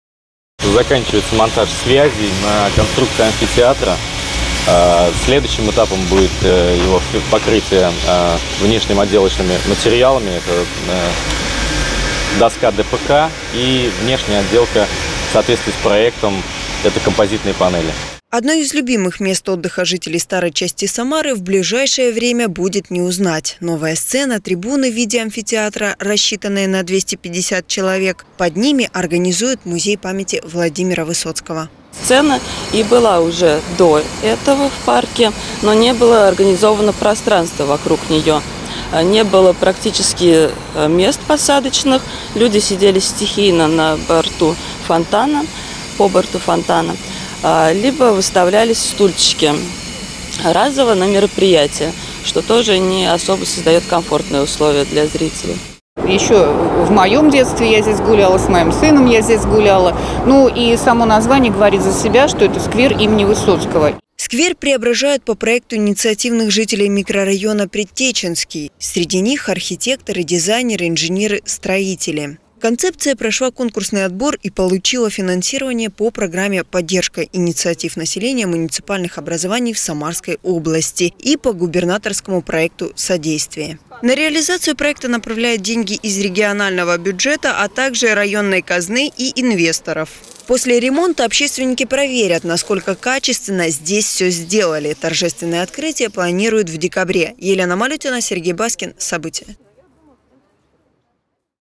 Одно из любимых мест отдыха жителей Самарского района — сквер Высоцкого — приведут в порядок
Новая сцена, трибуны в виде амфитеатра,  музей -памяти Владимира Высоцкого. Одно из любимых мест отдыха жителей  Самарского района - сквер Высоцкого приведут в порядок.  Когда самарцы смогут придти в обновленный парк  выяснила Елена Малютина.